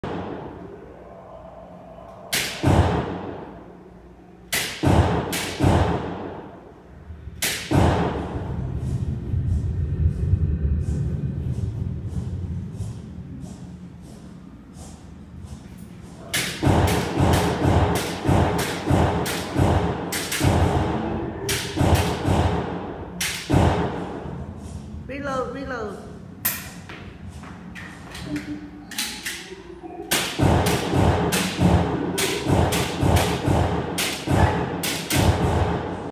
Valor Defense Academy - Zombie Apocalypse games
Fun stress relief for COVID-19... Bring your own group to shoot dozens of human sized ZOMBIES on the huge 32' x 7' HD 4K theater screen, using real OEM Glock 19 Gen4/5!!! 👿👻👹🔫🔫🤠😁🤣😂

Valor Defense Academy - Simulation Shooting Range - Beyond Target Shooting